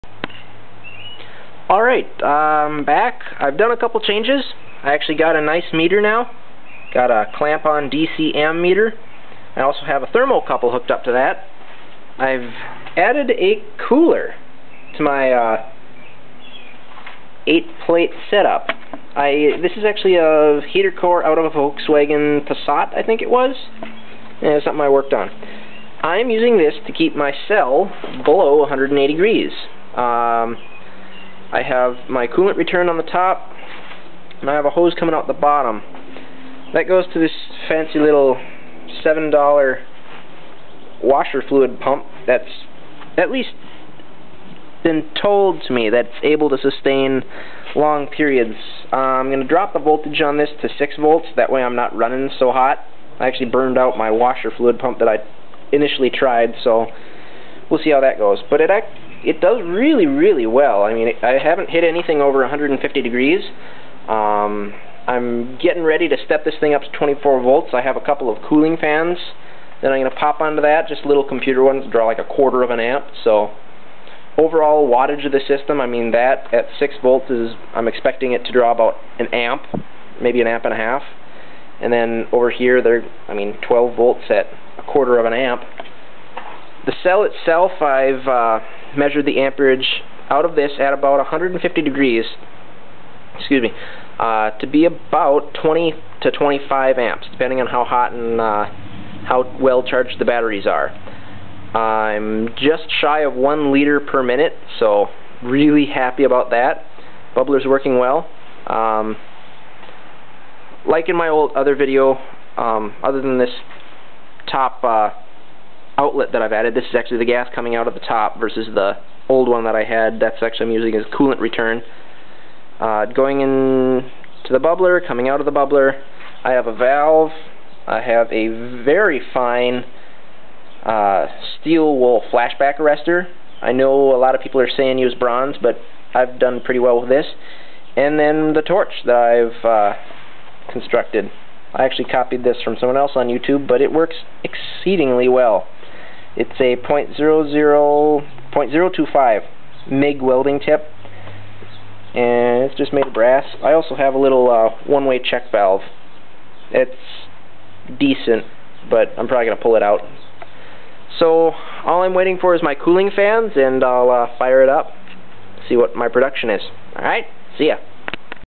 New design for HHO cell
I've re-built the cell with 2 packs of 4 plates, total arrangement being -.n.n.+.+.n.n.- I'm drawing 20-25 amps at 24volts, running at 150 degrees. I have a new torch head that has been fired up and works well! I want to get 36volts in this to see if I can break 1L/min. I am using Baking soda as my electrolyte, 1 teaspoon per gallon of distilled water. Home Depot 302 stainless plates. 3" PVC body for cell, 2" pvc for bubbler.